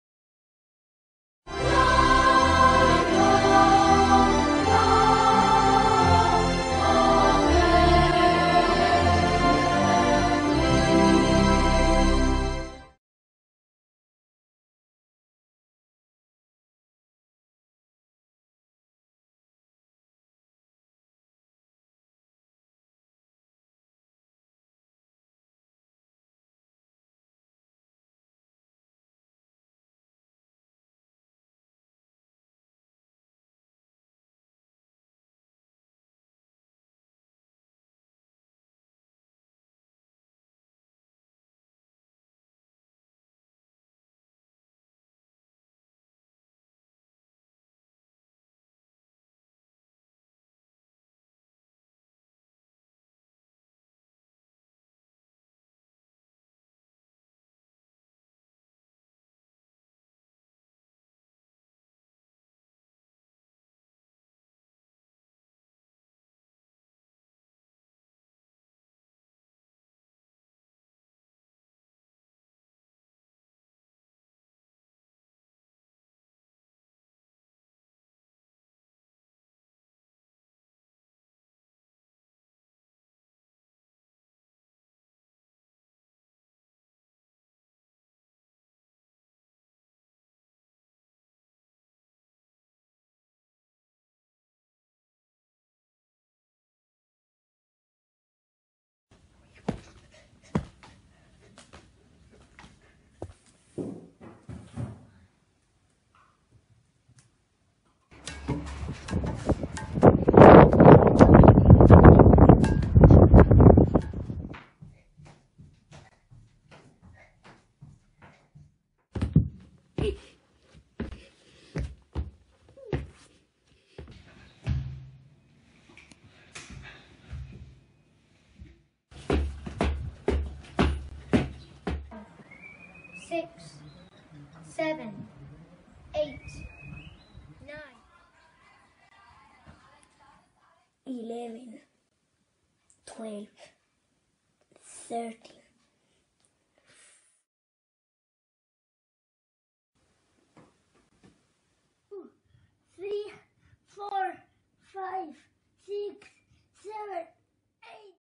ECI PE ONLINE
Pupils doing their PE lessons online as part of ECI ONLINE